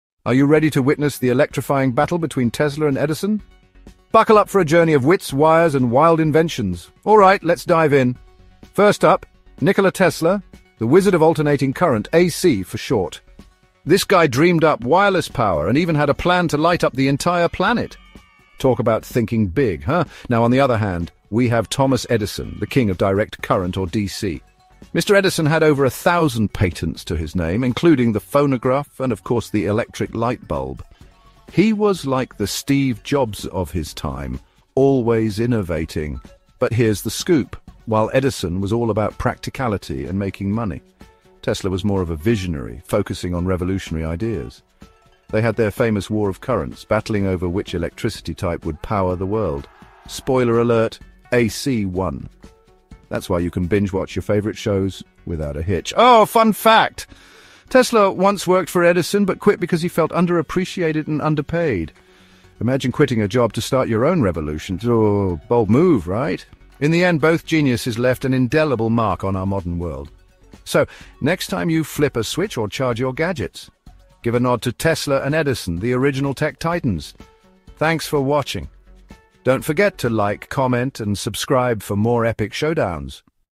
Tesla vs Edison ⚡funfacts
Edison invented the lightbulb, but Tesla lit up the world! Their rivalry over electricity changed history forever. 🔌⚔️